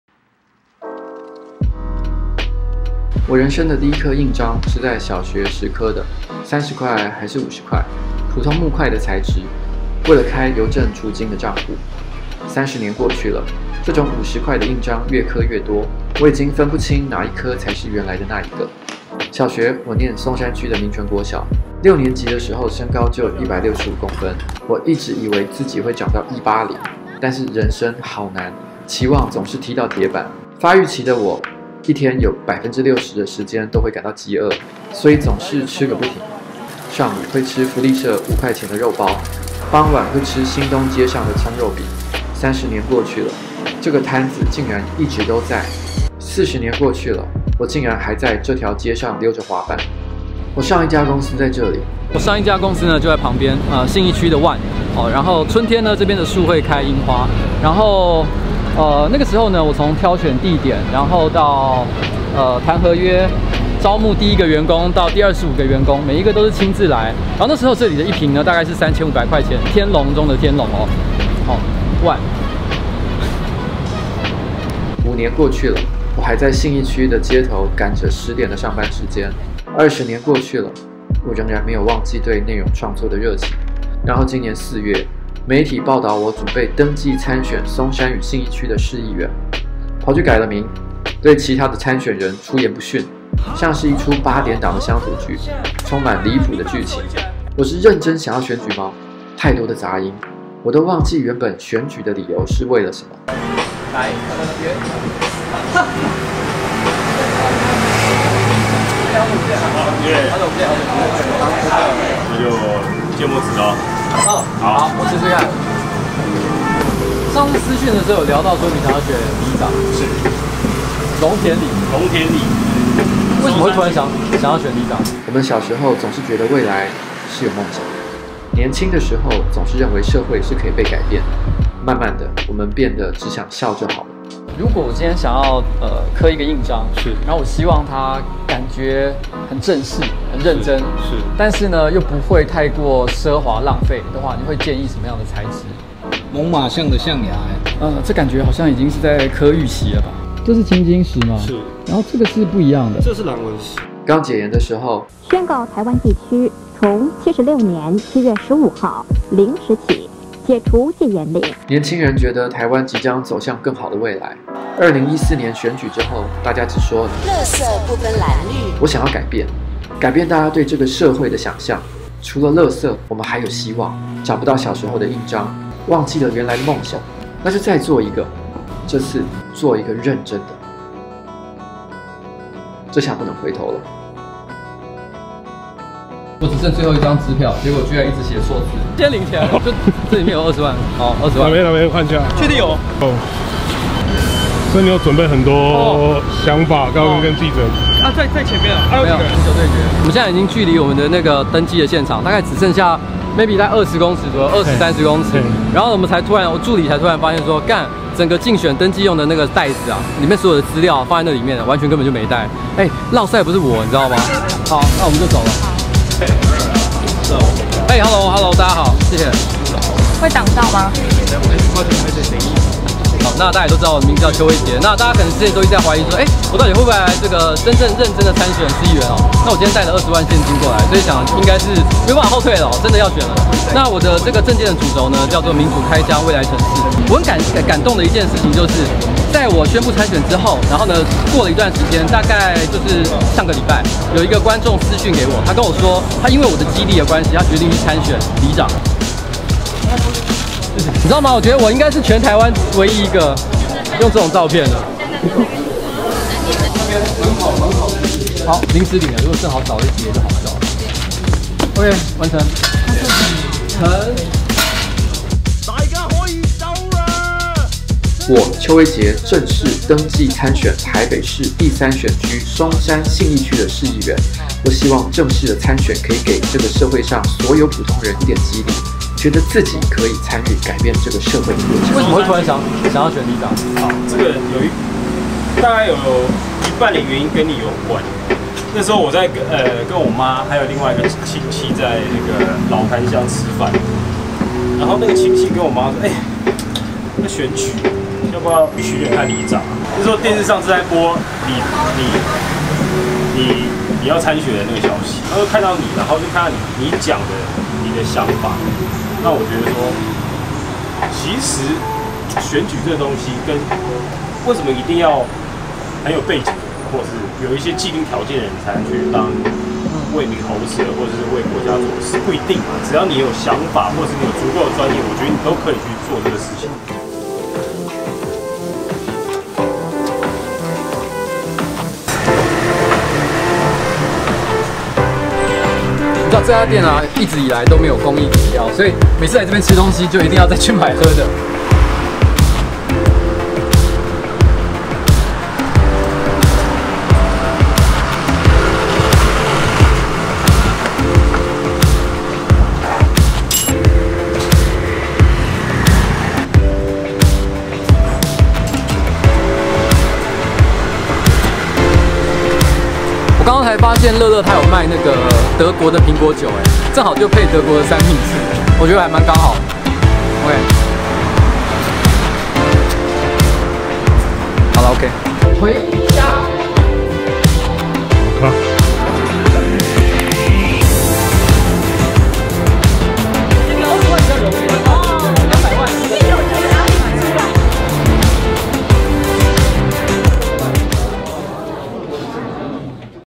【呱吉】呱吉登記參選市議員那一天到底發生了什麼事
因為有你們的參與，這一切才有可能

謝謝
等等我發現我最後的印章廠商名稱寫錯了，感謝「橙印良品」贊助我那顆印章。

我真的對他們感到很抱歉，所以為了表示歉意，罰寫十遍

【橙印良品專業製印所】